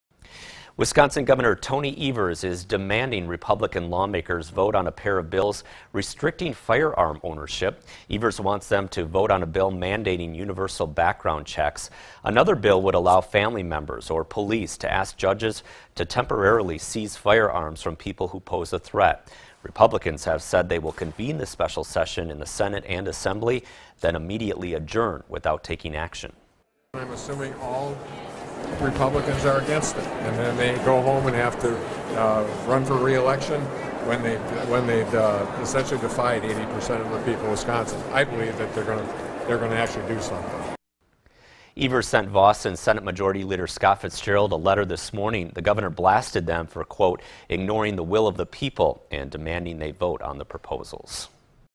Wisconsin GOP to dodge governor's call for gun control bills
Wisconsin Republicans who control the state Legislature were expected to dodge the Democratic governor's call to pass a pair of gun control bills in a special session Thursday, even as advocates planned to up the pressure on them by holding a rally and flooding the galleries.